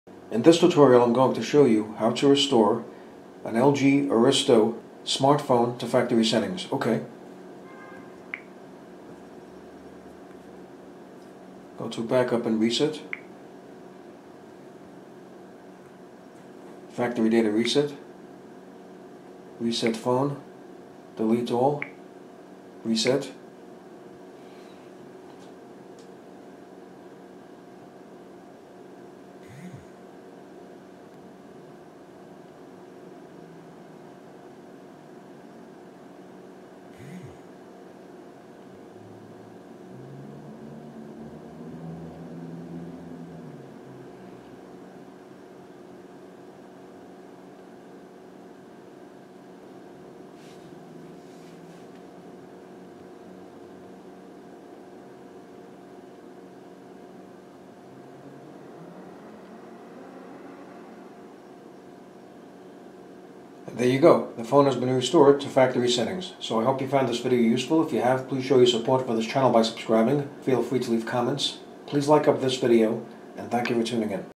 How To Restore An LG Aristo MS210 Smartphone To Factory Settings
Data wipe / erase restore to factory settings tutorial for the LG Aristo MS210 smartphone.